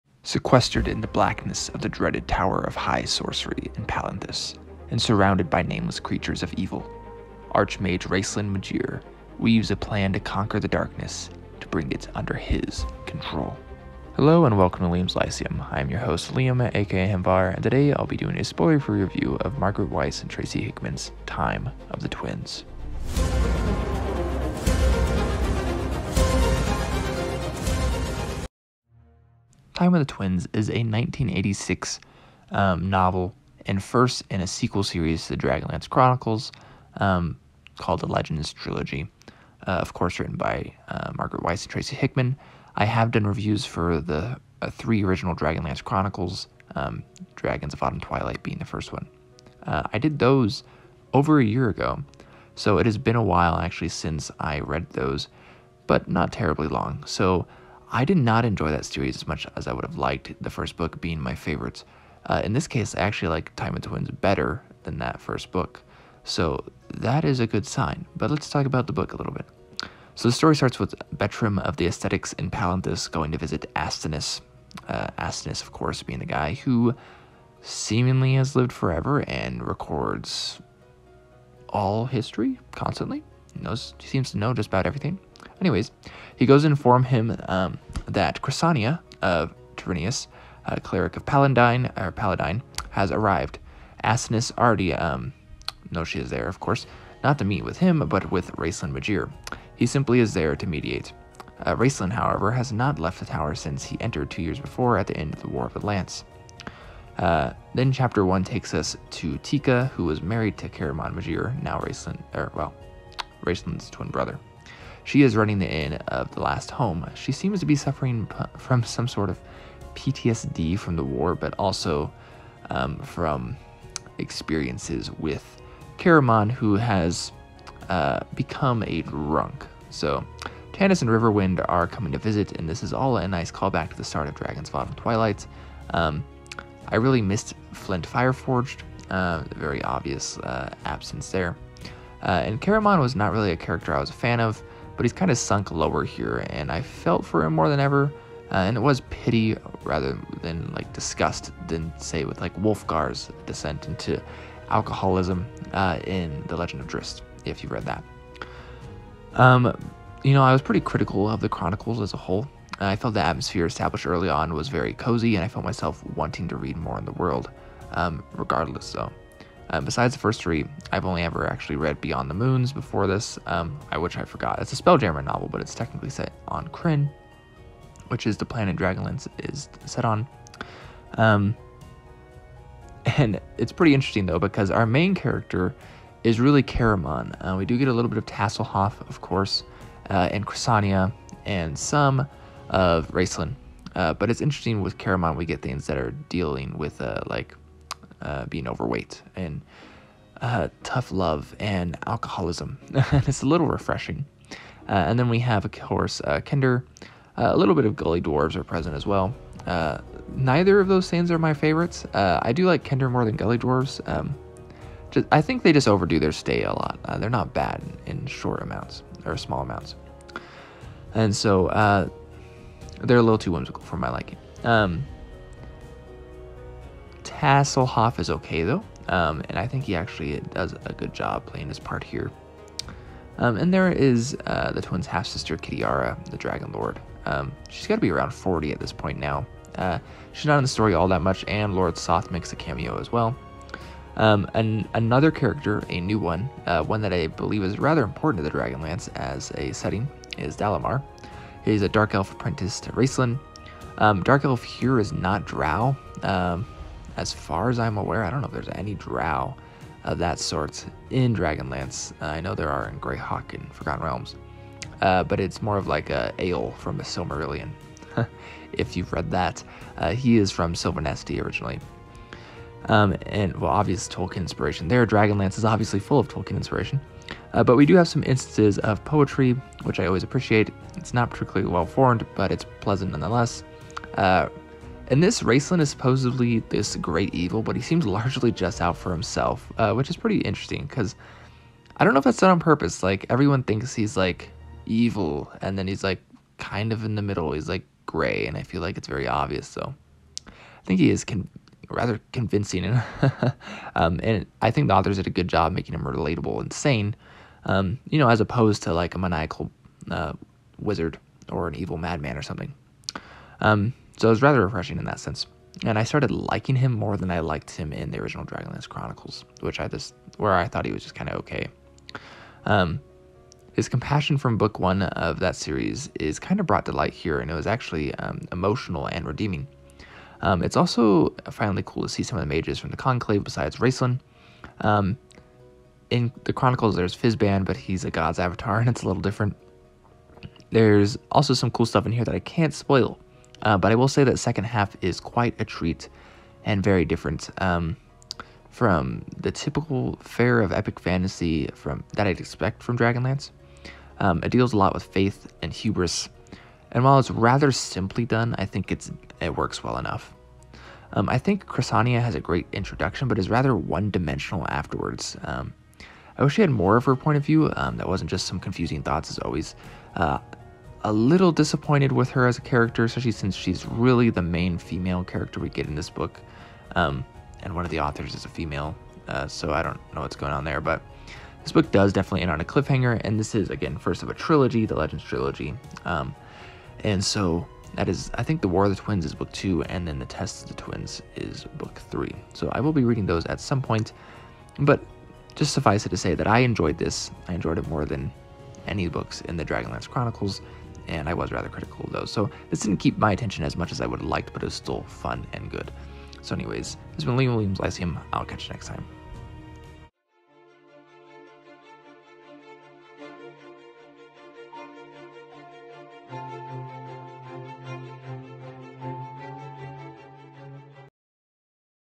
Time of the Twins by Margaret Weis and Tracy Hickman | Spoiler Free Review
Margaret Weis and Tracy Hickman bring the first of The Legends Trilogy I’m the world of Dragonlance. What is Raistlin up to now, and what will Caramon do to stop him?

Intro:
Music from Uppbeat (free for Creators!):